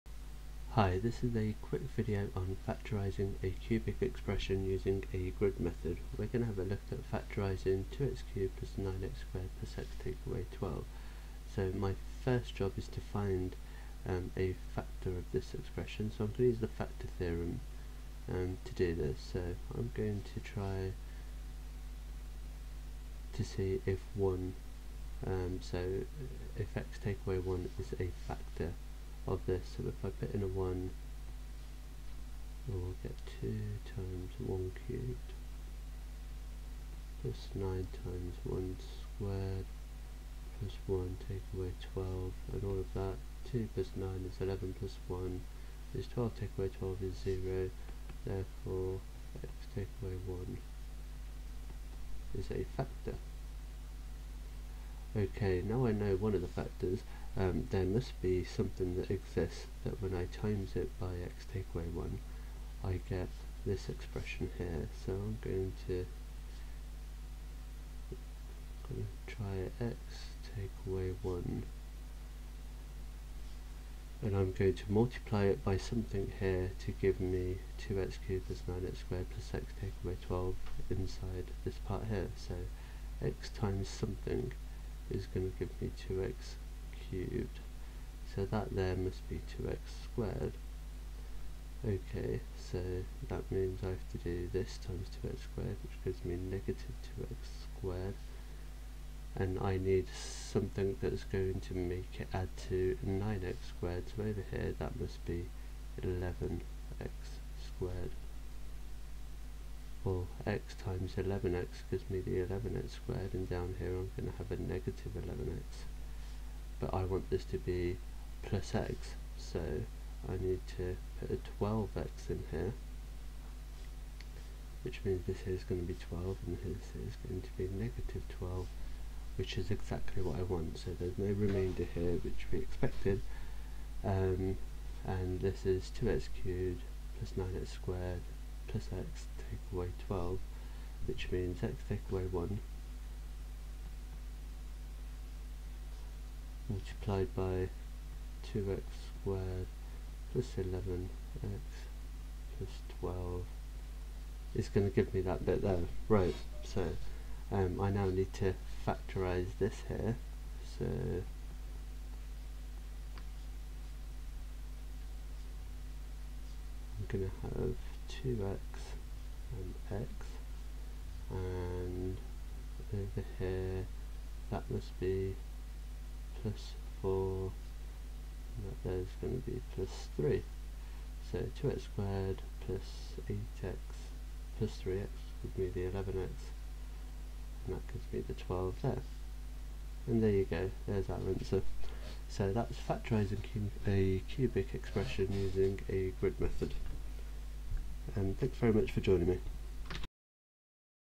Factorising a cubic expression
Factorising a cubic expression using a grid method instead of long division.